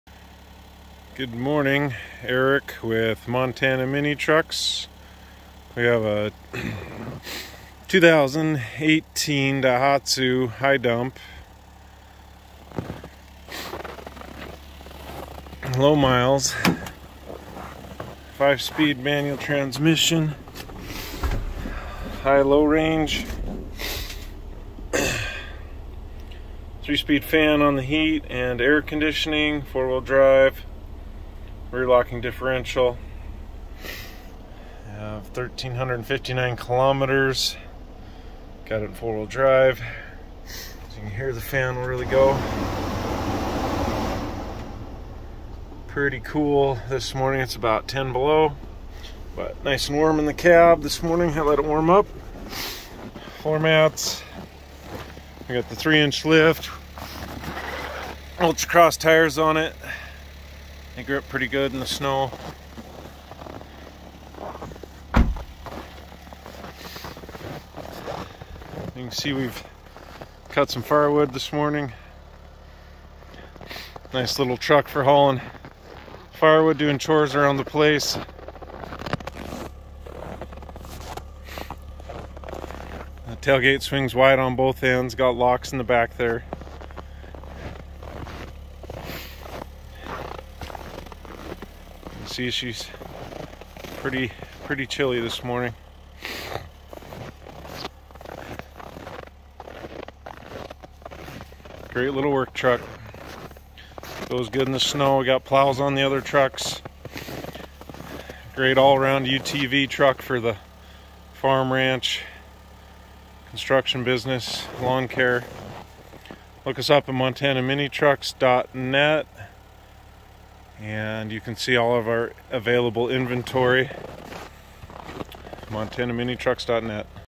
2018 Daihatsu high dump for sale!Montanaminitrucks.net
Montana Mini Trucks are the most reliable UTV/ATV on the market.  3 inch lift with1355 km/841 miles. 4x4 hi/Low range. 5 speed manual work horse! Check us out at montanaminitrucks.net